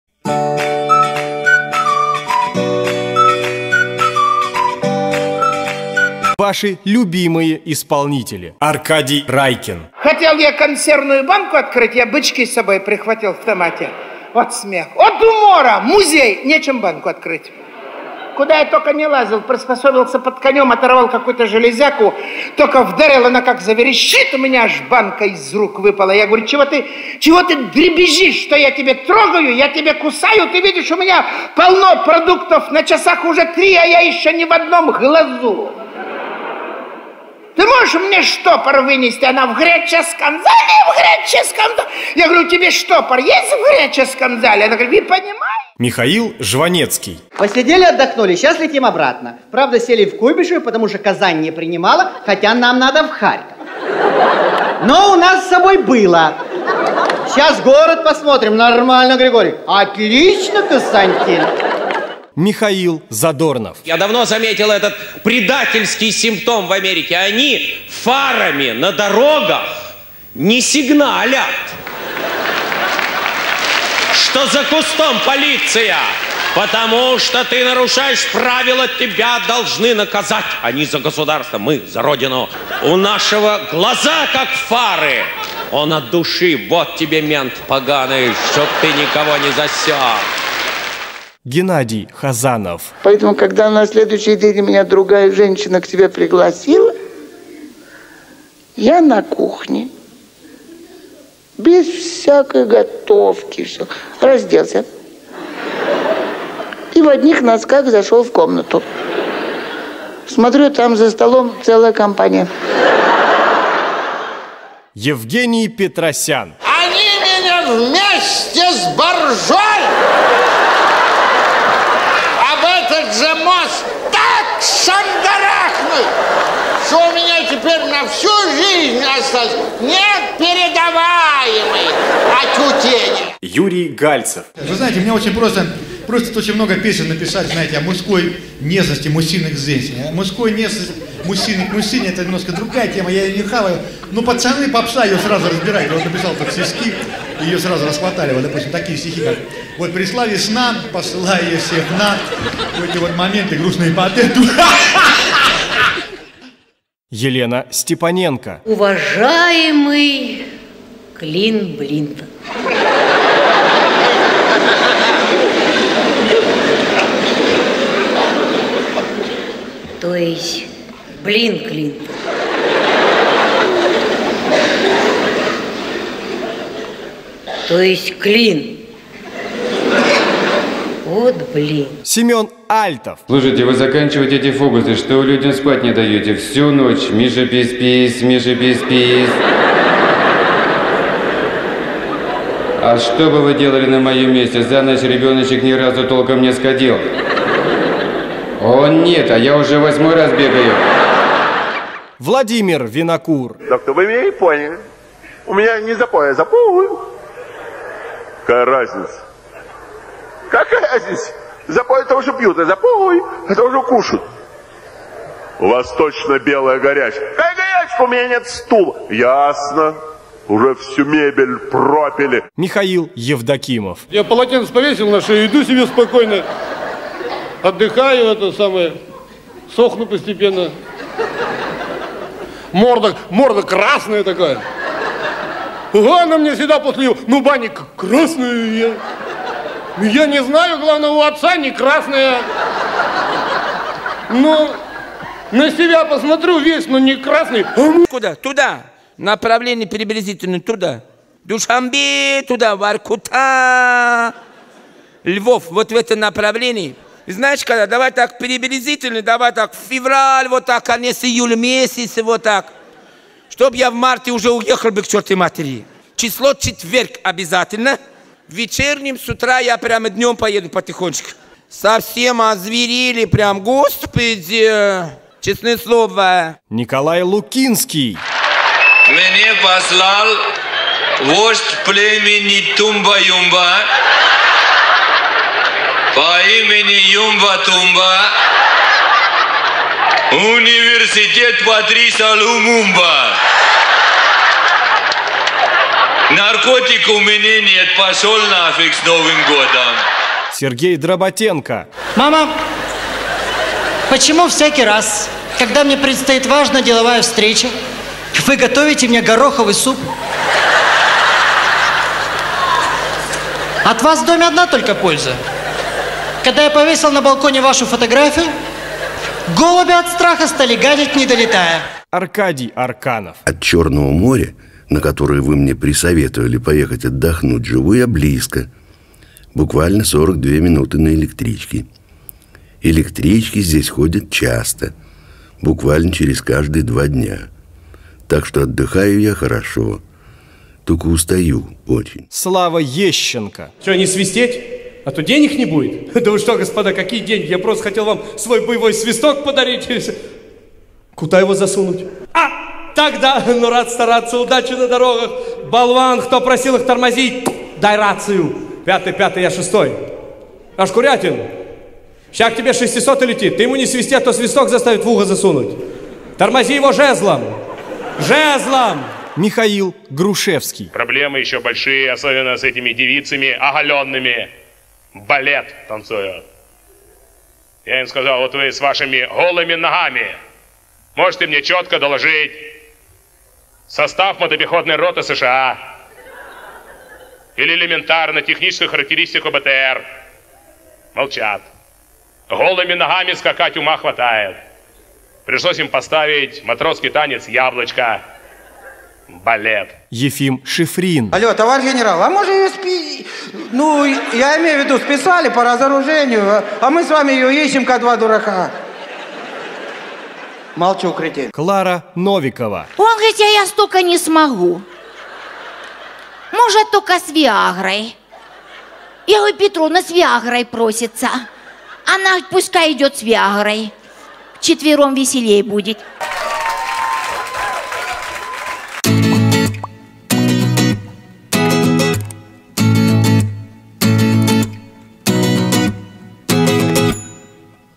Ваши любимые исполнители (Юмор смех пародия)
Ваши любимые исполнители (Юмор смех пародия)                                                                               Содержания:                                                                                                                                                             Аркадий Райкин
Михаил Жванецкий
Михаил Задорнов
Геннадий Хазанов
Евгений Петросян
Юрий Гальцев
Елена Степаненко
Семён Альтов
Владимир Винокур
Михаил Евдокимов
Ян Арлазоров
Николай Лукинский
Сергей Дроботенко
Аркадий Арканов
Святослав Ещенко
Михаил Грушевский
Ефим Шифрин
Клара Новикова
🔴 Юрий Гальцев и Елена Воробей Рядовой сержант Сокол Воробей